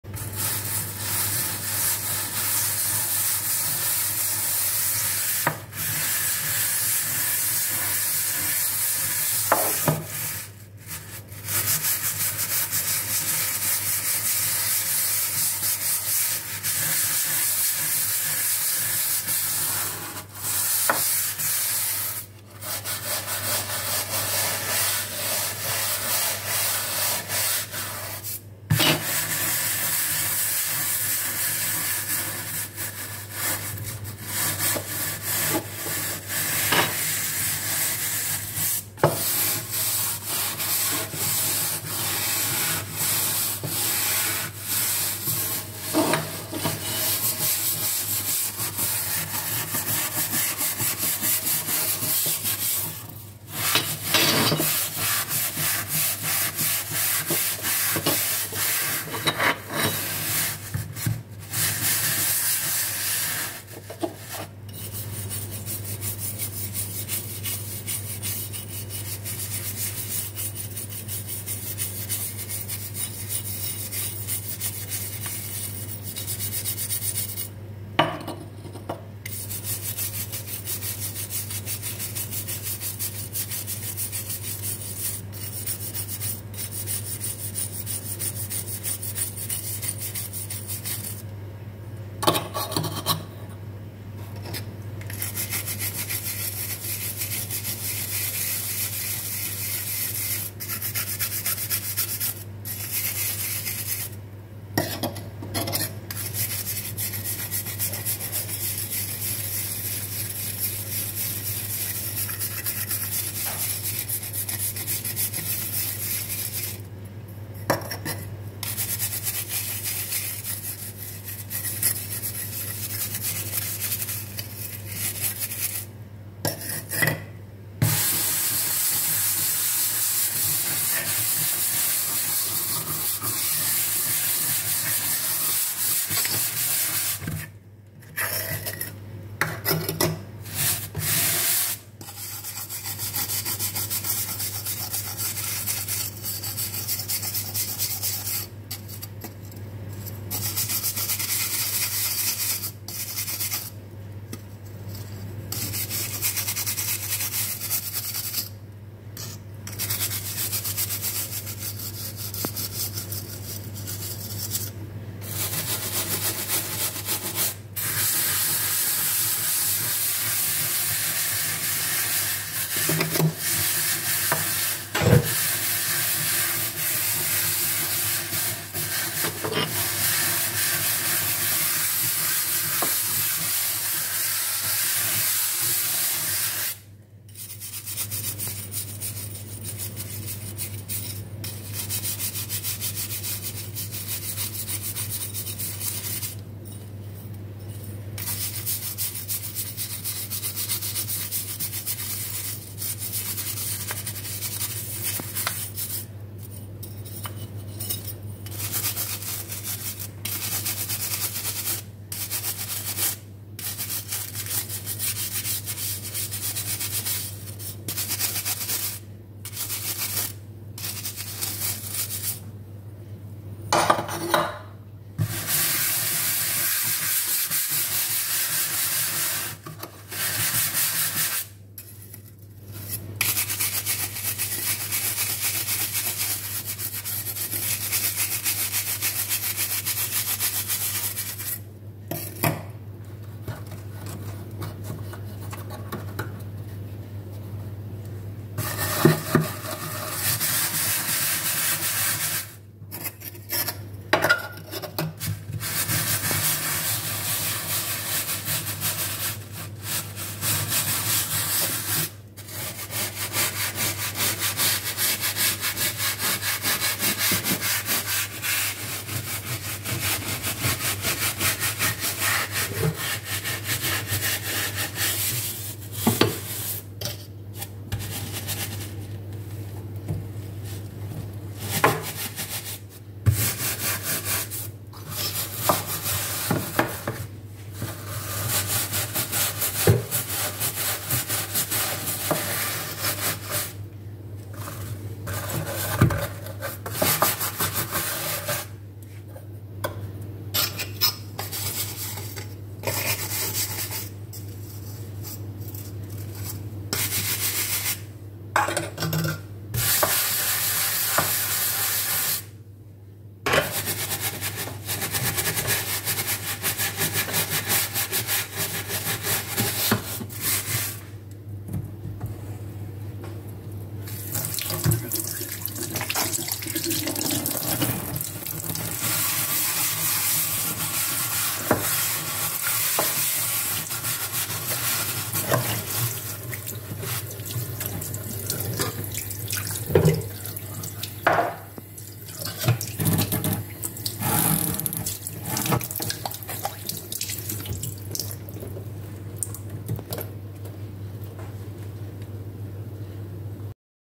ASMR | Cleaning my sink 🧽
I'm not crazy about cleaning, nor a big fan of cleaning from top to bottom, but when it's necessary, you have to get started